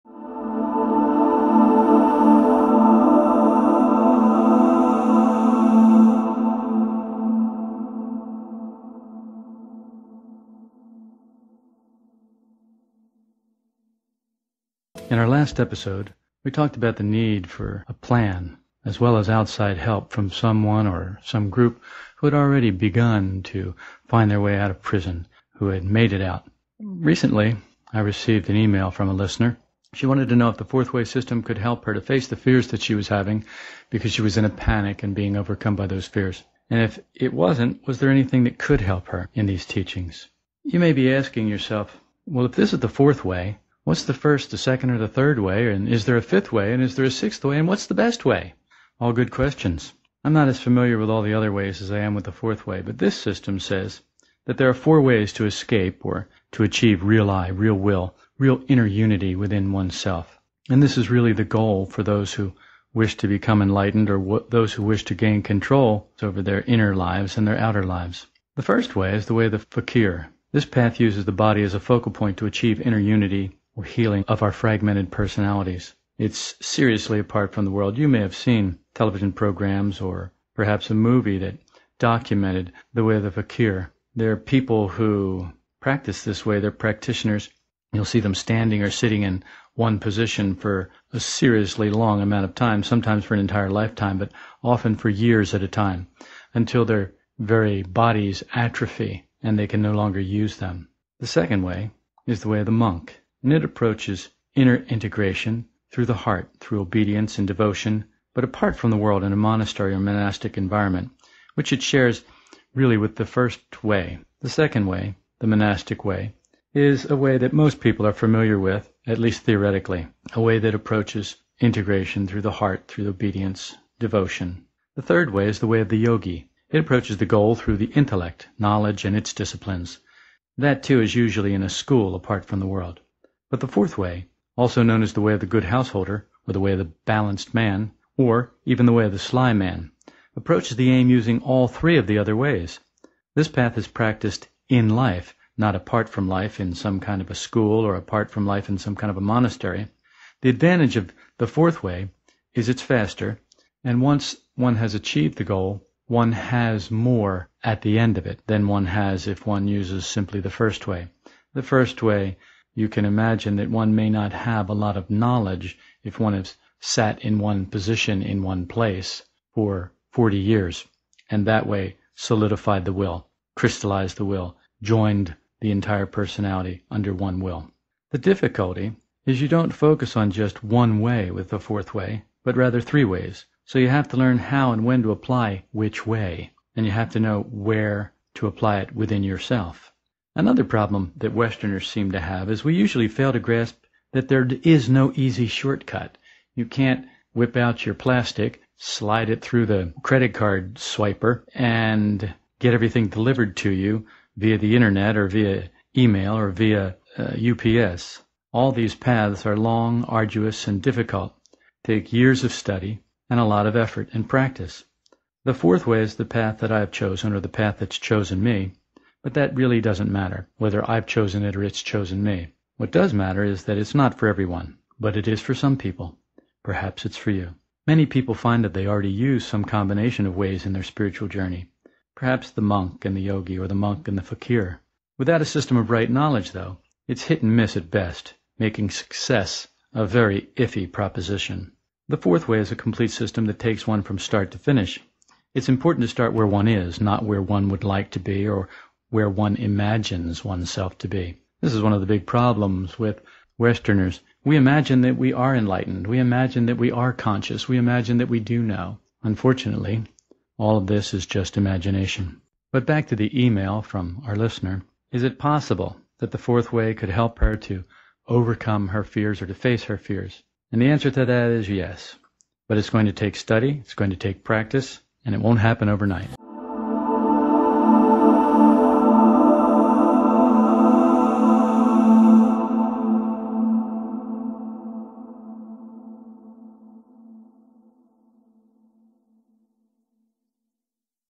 Four Ways | Esoteric Talks
An email question from a listener leads us to talk about the Four Ways to awakening or enlightenment.The Way of the Fakir, The Way of the Monk, the way of the Yogi and The Way of the Balanced Man.